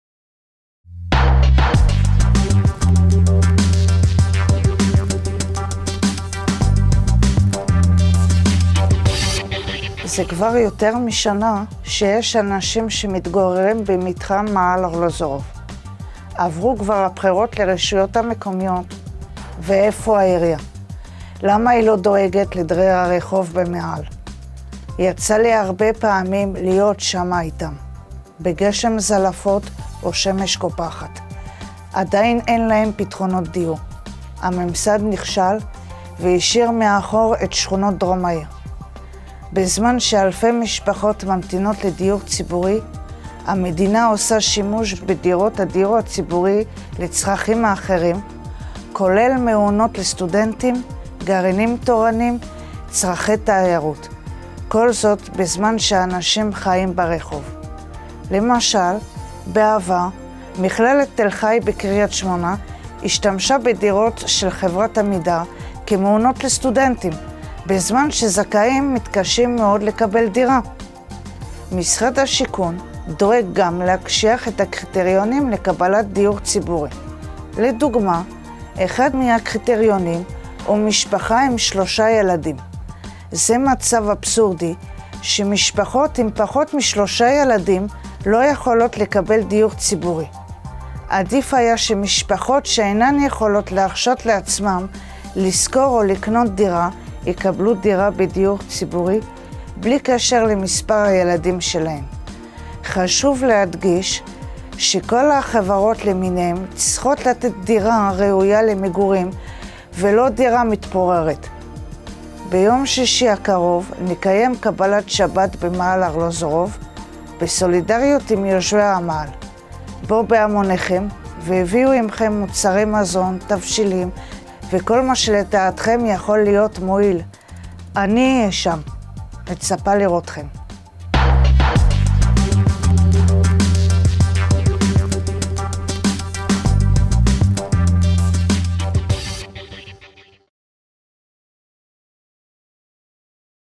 רבקה ויטנברג - מאהל מחוסרי דיור
מיקרופון לדמוקרטיה בטלוויזיה החברתית - בעלי מסר אקטואלי, מקבלים קול ובמה להעברת מסרים חדים לחיזוק הדמוקרטיה. הפעם רבקה ויטנברג, פעילה חברתית ופוליטית, על כישלון הממסד בסיפוק דיור ציבורי לזכאים וקריאה להצטרפות לקבלת שבת במאהל ארלוזרוב תל אביב, בסולידאריות עם יושבי המאהל.